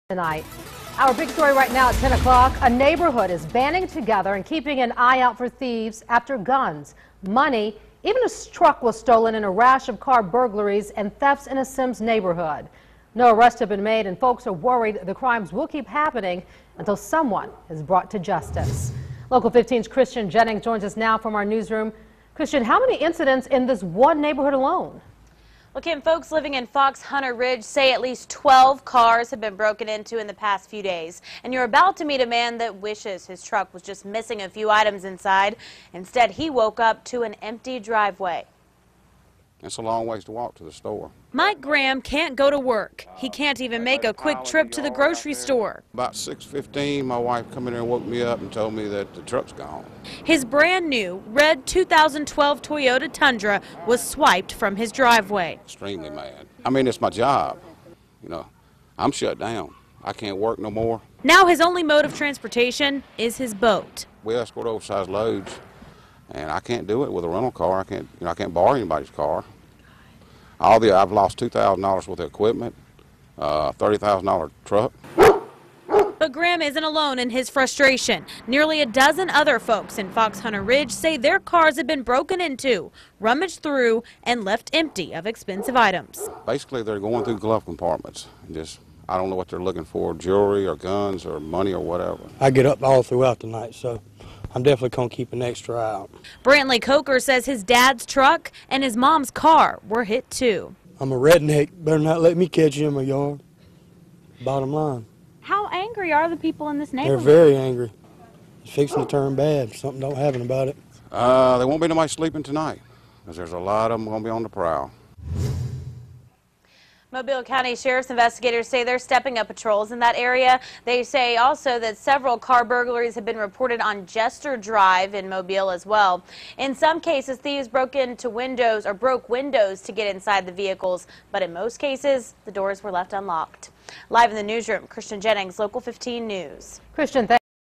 Cars Broken Into, One Stolen In Semmes
SEMMES, Ala. (WPMI) A neighborhood is banding together and keeping an eye out for thieves after guns, money and even a truck was stolen in a rash of car burglaries and thefts in the Semmes neighborhood, Fox Hunter Ridge.  No arrests have been made and folks are worried the crimes will keep happening until someone is brought to justice.
It's a long ways to walk to the store, said Mike Graham, who now can't work or even make a quick trip to the grocery store.
About 6:15 this morning my wife come in there and woke me up saying the truck is gone, said Graham.
His brand new red 2012 Toyota Tundra was swiped from his driveway.
I'm extremely mad. I mean it's my job. I'm shut down. I can't work no more. We escort oversized loads and I can't do it with a rental car. I can't borrow anyone's car. I've lost $2,000 worth of equipment and a $30,000 truck, he said.
But Graham isn't alone in his frustration. Nearly a dozen other folks in Fox Hunter Ridge said their cars have been broken into, rummaged through and left empty of expensive items.
Basically they're going through glove compartments. I don't know what they're looking for: Jewelry or guns or money or whatever, said Graham.
I get up all through the night so I'm definitely going to keep an extra eye out, said Brantley Coaker, who also lives in Fox Hunter Ridge.
He said his dad's truck and his mom's car were hit too.
How angry are the people in this neighborhood? asked Local 15's Christian Jennings.
They're very angry. Something bad is going to happen if something isn't done about it, said Coaker.
There won't be anybody sleeping tonight 'cause a lot will be on the prowl. I mean we have no crime in here. This is the first time we've had something in over a year, said Graham.
Mobile County Sheriff's investigators said they're stepping up patrols in the area. They said several car burglaries have been reported on Jester Drive as well.
In some cases, thieves broke windows to get inside the vehicles. But in most cases deputies said the car doors were left unlocked.